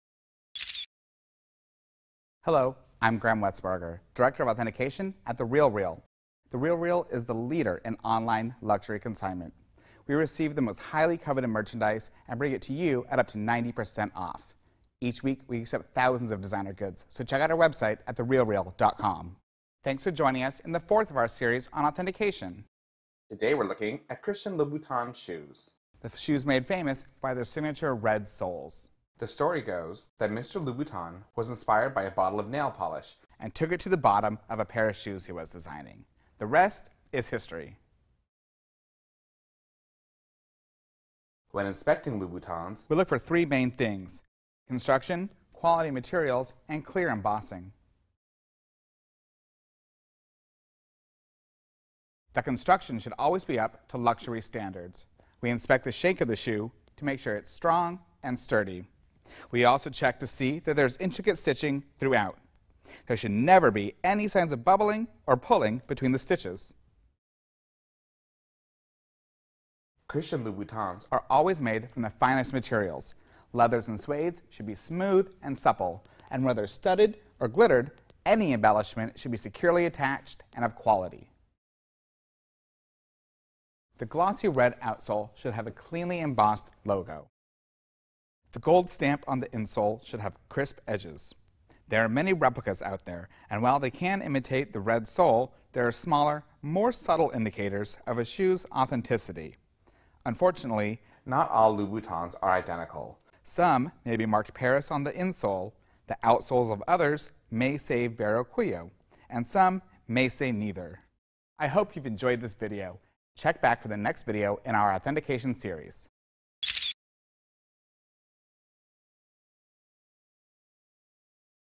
Cách phân biệt giày Christian Louboutin 'nhái'
Các tín đồ nên chú ý đến cấu trúc, chất liệu cũng như chữ dập nổi ở mặt dưới của giày.
 Bạn bị yếu sinh lý xem ngay cách chữa